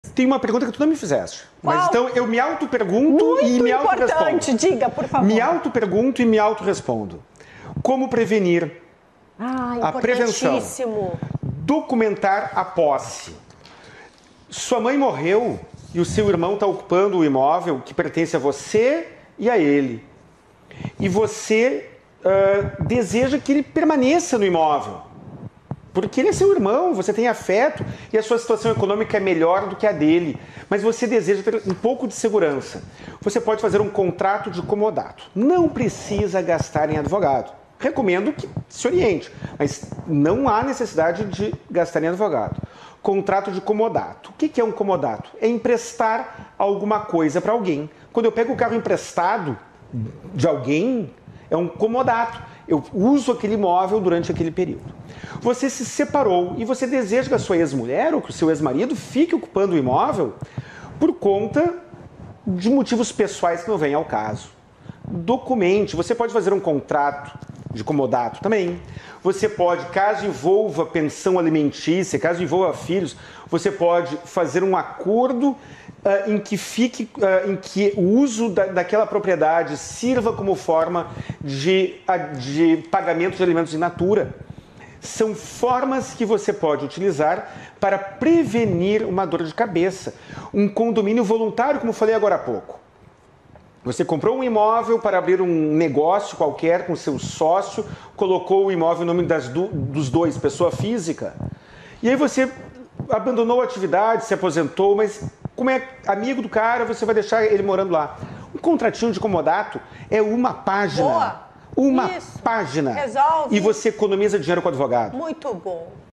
Prevenção contra o Usupiao Traiçoeiro
Recorte do vídeo - Cobrar Aluguel de Imóvel com mais de um proprietário

Em casos de Condomínio Voluntário, ou seja, quando o mesmo imóvel tem mais de um proprietário, seja porque eles compraram o imóvel em conjunto, seja por conta de uma partilha de bens em herança ou separação judicial, caso um dos proprietários siga utilizando sozinho o imóvel, o outro terá o direito de exigir uma indenização a título de aluguel.

Também há o risco de usucapião, isso inclui os casos de imóvel utilizado pelo cônjuge após a separação de fato, e do herdeiro que utiliza sozinho o imovel.

Guilherme Collin
OAB/RS 48.682

Fones: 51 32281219 / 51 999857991